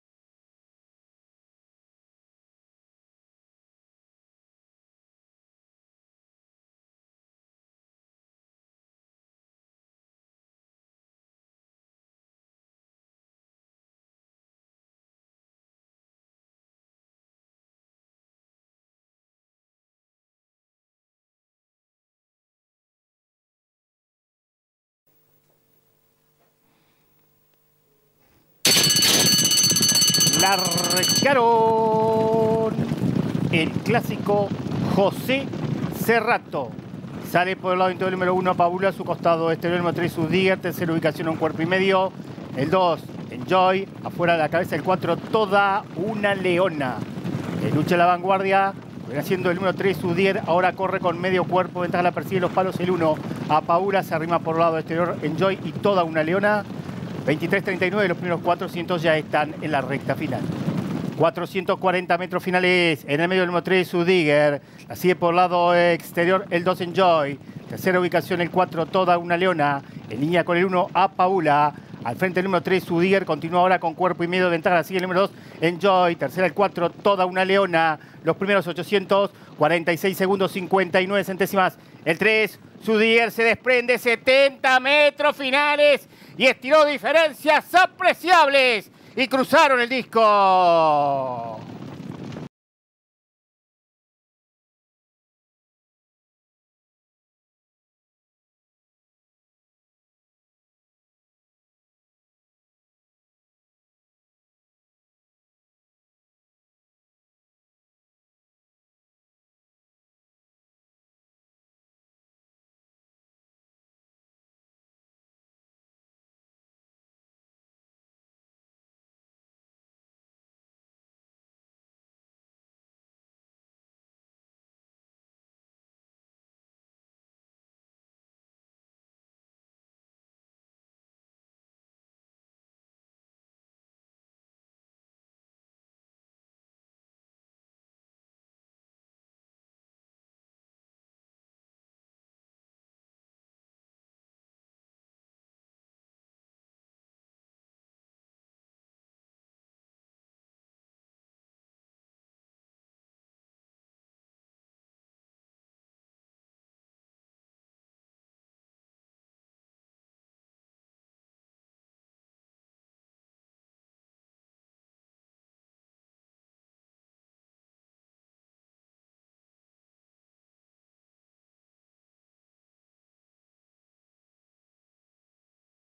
25/01/2015 – Hipodromo Maroñas – Carrera 5 – JOSE SERRATO
Carrera 5 realizada el dia 25/01/2015 por el premio JOSE SERRATO en Hipodromo Maroñas